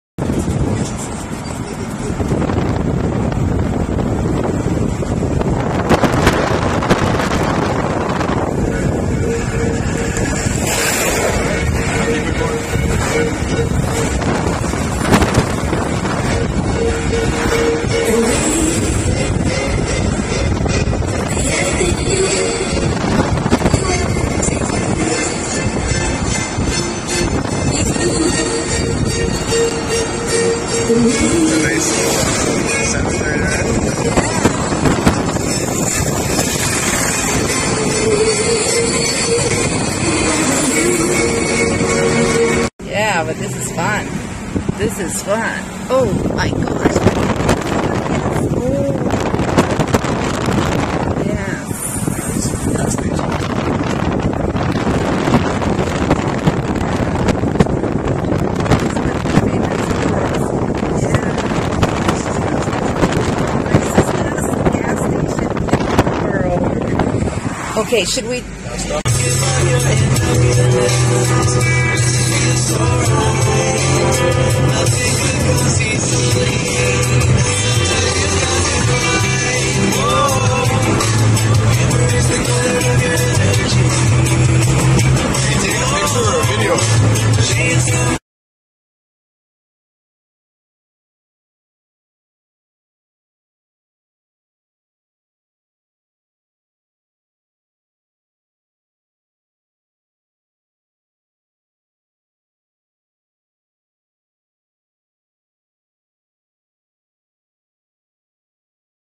Caribbean Vacation Grand Cayman Diana Walker's Movie
Caribbean Vacation dianawalker.com Grand Cayman Diana Walker visiting son Jesse Walker Beautiful blue ocean, beautiful sky and water... Travel Love it!